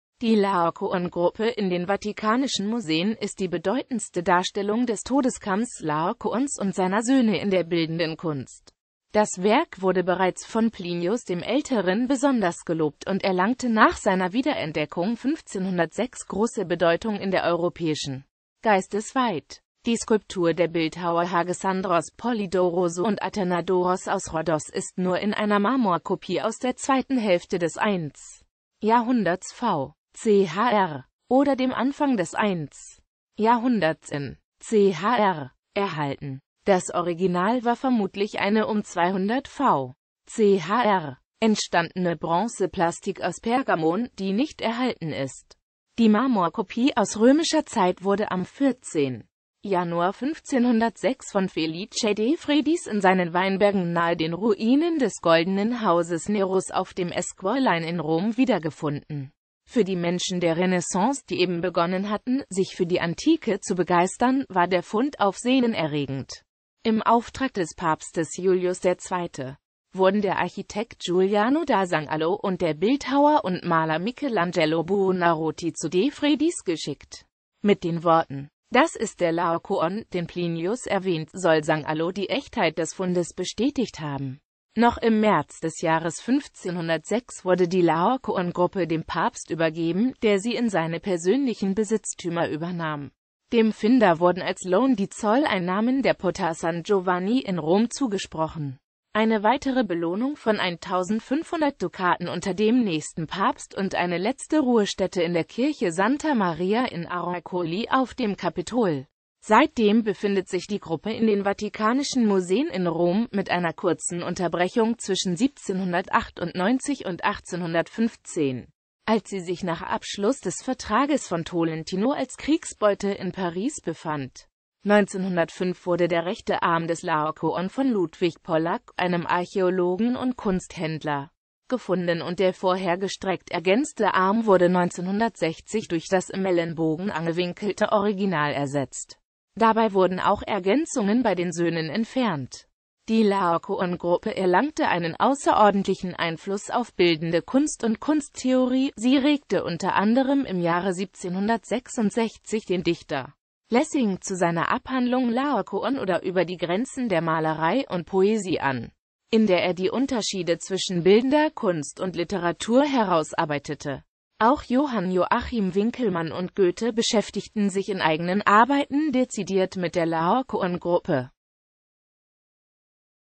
Laokoon-Gruppe
Vielen Dank für Ihre Unterstützung:

Die Laokoon-Gruppe in den Vatikanischen Museen ist die bedeutendste Darstellung des Todeskampfs Laokoons und seiner Söhne in der bildenden Kunst.Das Werk wurde bereits von Plinius dem Älteren besonders gelobt und erlangte nach seiner Wiederentdeckung 1506 große Bedeutung in der europäischen Geisteswelt.Die Skulptur der Bildhauer Hagesandros, Polydoros und Athanadoros aus Rhodos ist nur in einer Marmorkopie aus der zweiten Hälfte des 1.Jahrhunderts v.

Bild-Copyright-Informationen
Urheber Info: Original uploader was Paul Barlow at en.wikipedia